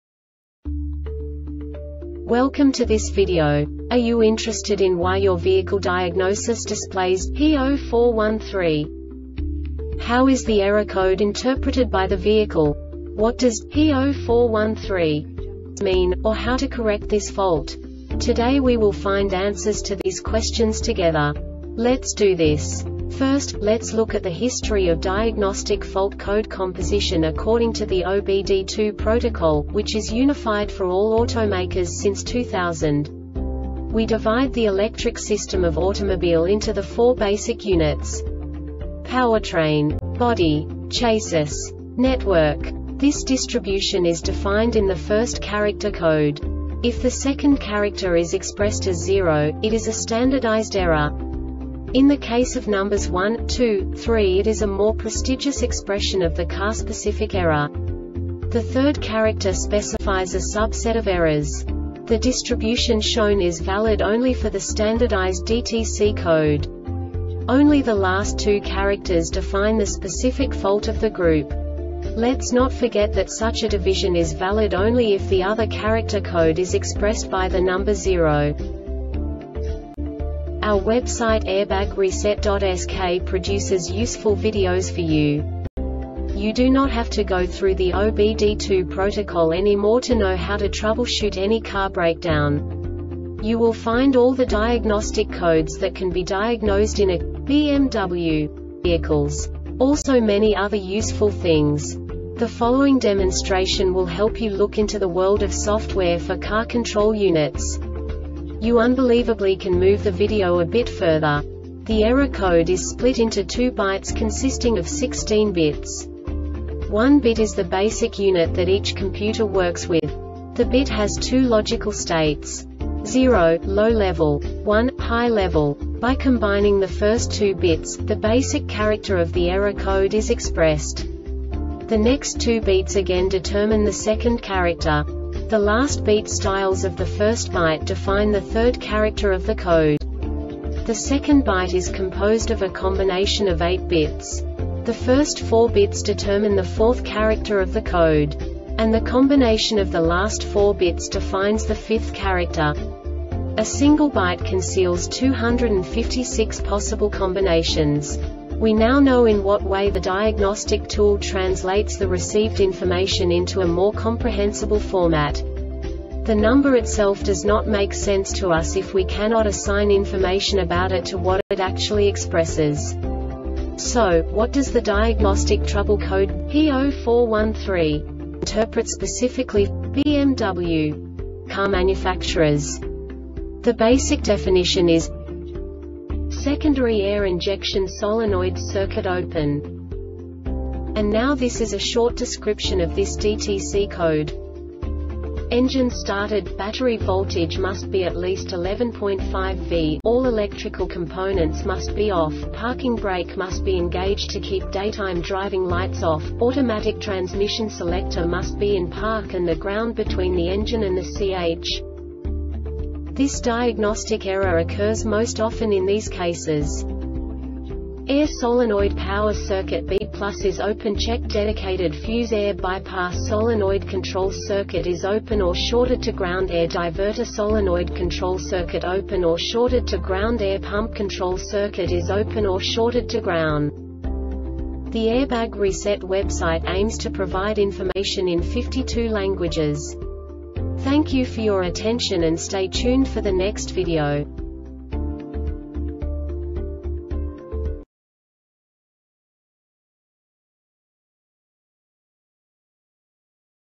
DTC BMW P0413 Short Explanation
Contents:
0:21 Basic DTC analysis according to OBD2 protocol standard.
1:48 Insight into programming
2:58 A diagnostically understandable description of the Diagnostic Trouble Code
3:05 Basic error definition

Make:
BMW
Code:
P0413
Definition:
Secondary Air Injection Solenoid Circuit Open
Description:
Engine started, battery voltage must be at least 11.5v, all electrical components must be off, parking brake must be engaged (to keep daytime driving lights off), automatic transmission selector must be in park and the ground between the engine and the ch
Cause:
AIR solenoid power circuit (B+) is open (check dedicated fuse)
AIR bypass solenoid control circuit is open or shorted to ground
AIR diverter solenoid control circuit open or shorted to ground
AIR pump control circuit is open or shorted to groun